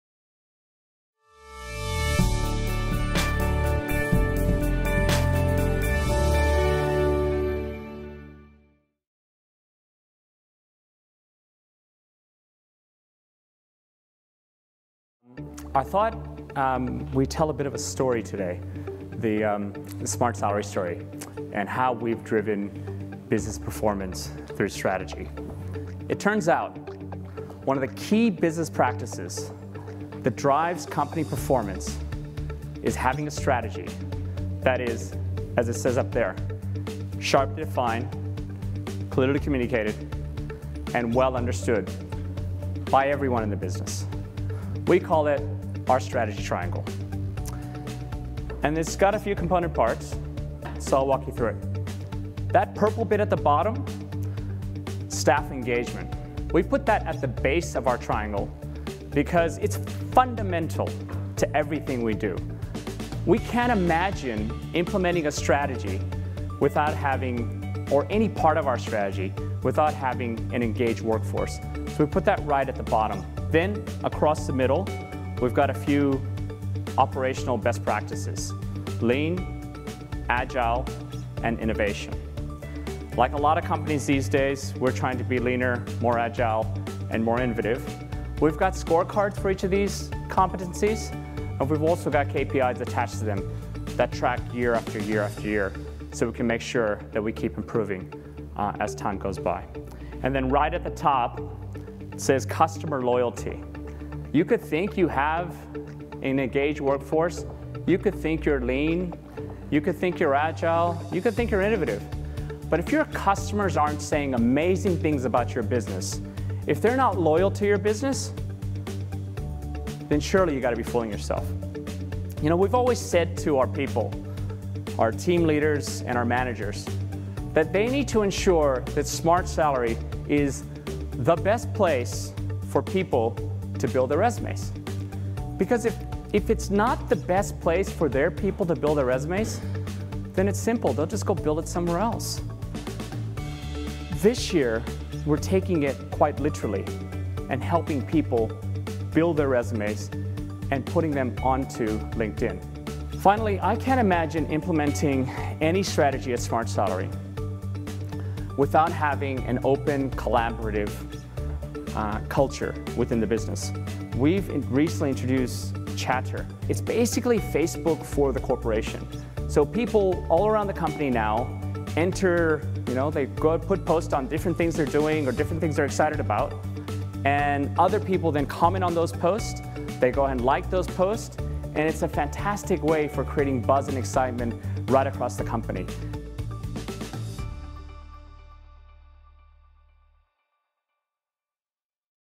Smartsalary CEO Deven Billimoria Speaks at CeBIT Australia
In May 2013, our CEO Deven Billimoria spoke at CeBIT in Sydney. Here what he had to say about how we use staff engagement to drive business performance.